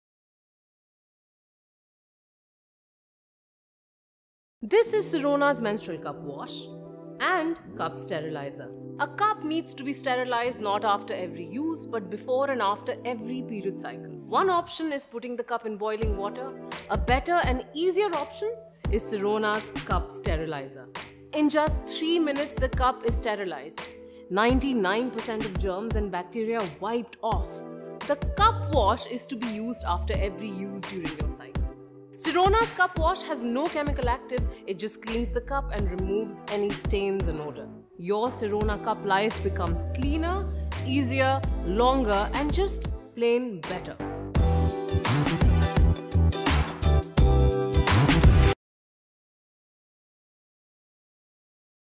Sirona Menstrual Cup Sterilizer Kills 99% of Germs in 3 Minutes with Steam
Specifications
Brand - Sirona
Type - Menstrual Cups Sterilizer

Product Description
Take control of your intimate & menstrual hygiene with Sirona Menstrual Cup Sterilizer. It kills up to 99% germs and bacteria in just 3 minutes; leaving your menstrual cup perfectly disinfected and ready to be stored till the next cycle.

FCid 3009277